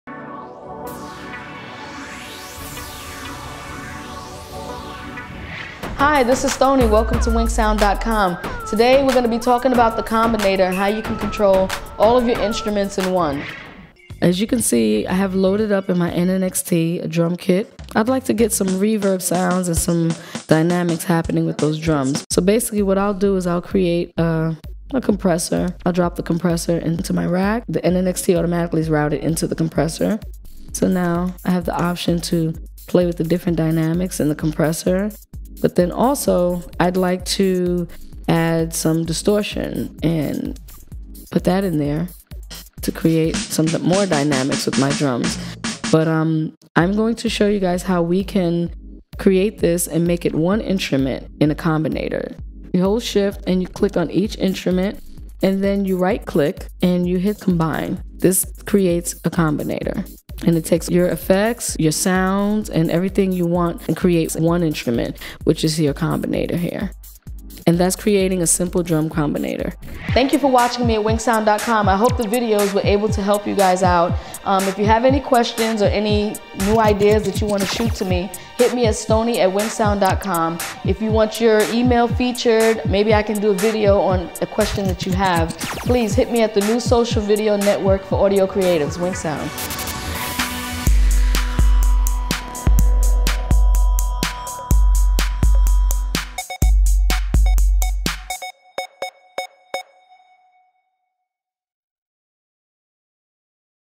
Propellerhead Reason: How to Create a Combinator | WinkSound
In this tutorial Stoni explains how to easily create a Combinator inside of Reason 4.

Keep up to date on the latest tutorials from WinkSound by following our feeds: